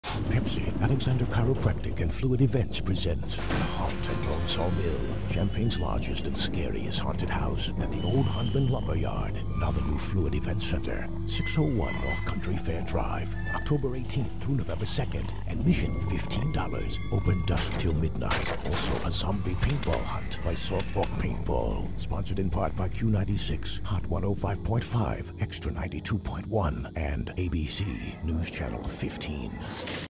The Haunt at Bone Saw Mill Commercial
The Haunt at Bone Saw Mill entails some of the most twisted and bloodcurdling visions of horror imaginable filled with spine-chilling denizens of the dark beyond. This is the Halloween destination that Champaign has been seeking. With over 30 distinct fright zones we are sure to hit upon the deepest fears of everyone that is brave enough to traverse our dark corridors.